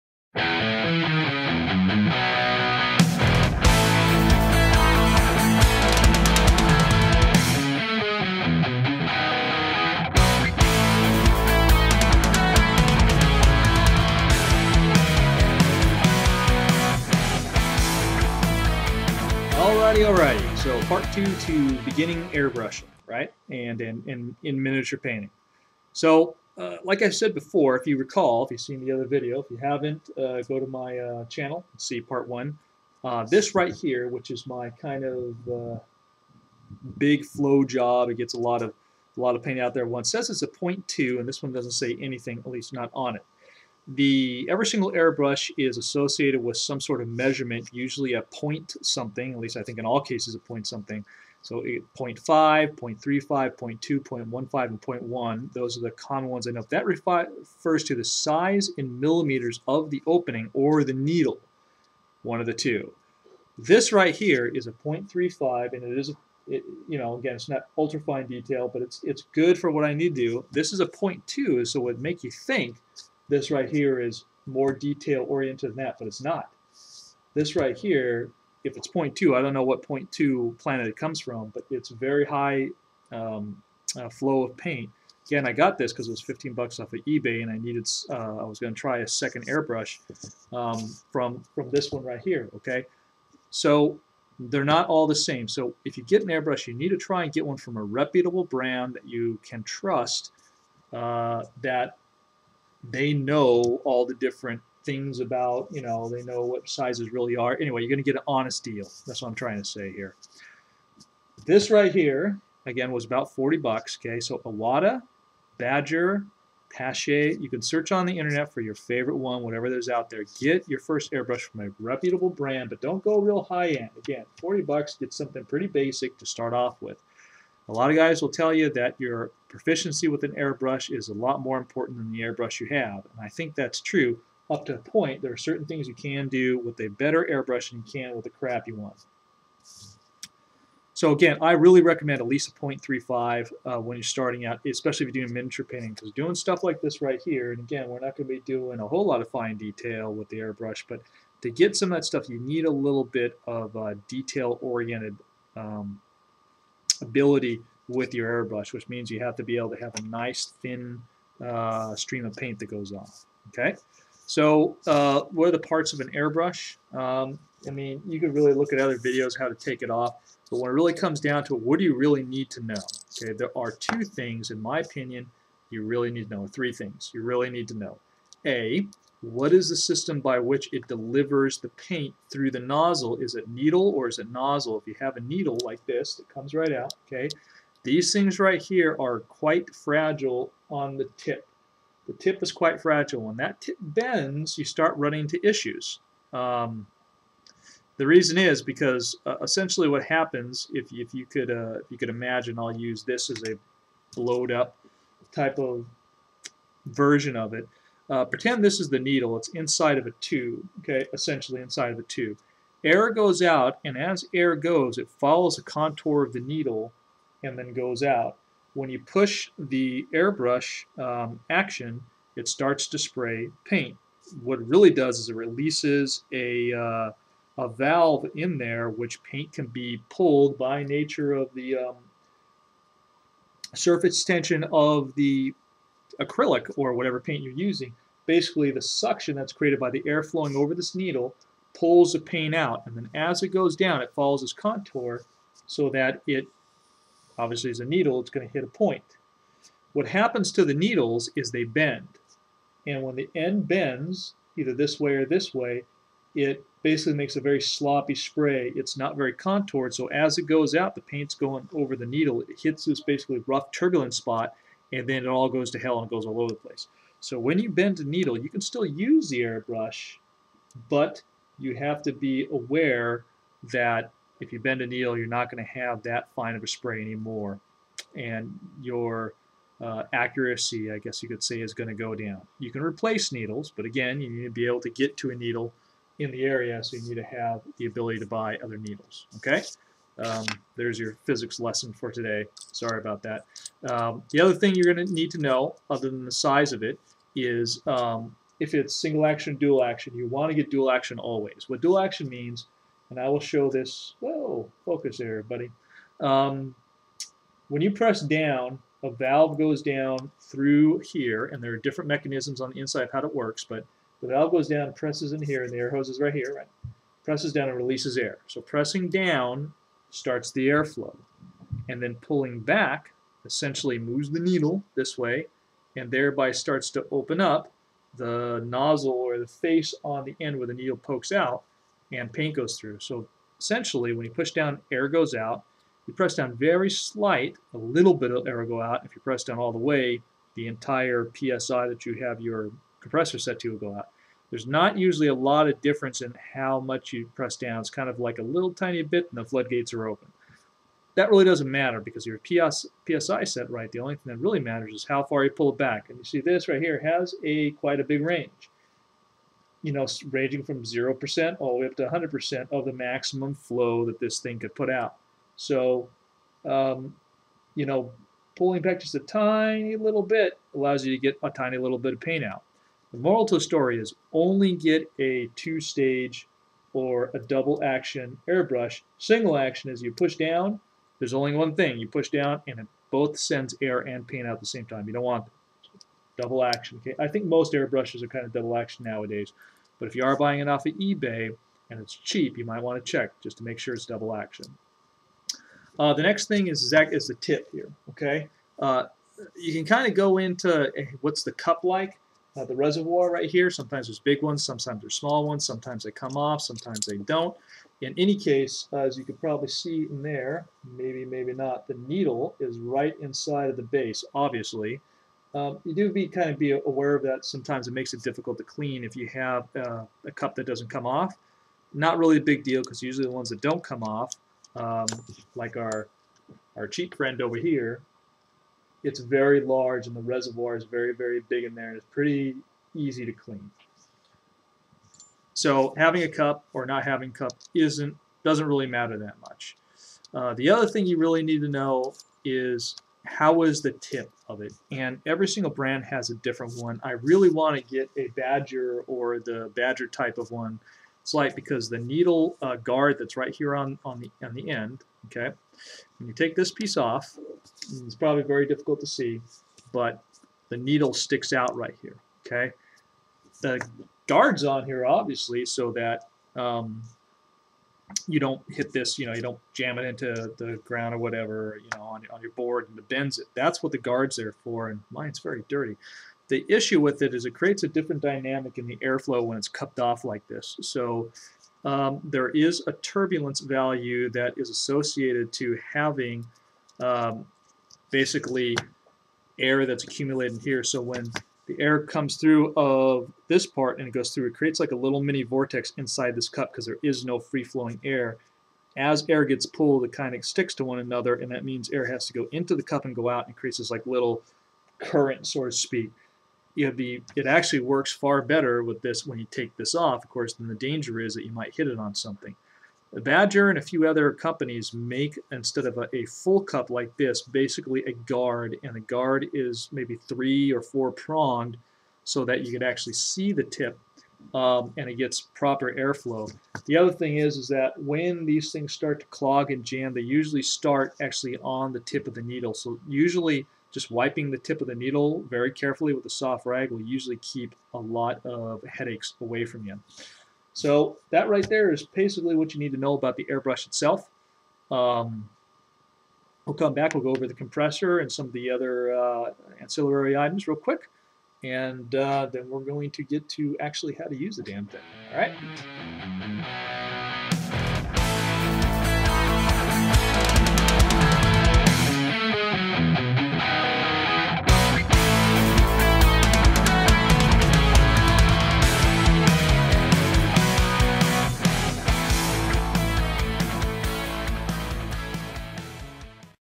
How to Airbrush for Beginners Part 2
A 7 part series on the very (VERY) basics of air brushing. The Hack shows you what you need, what the costs are, how to prep, the approach and actually some painting too!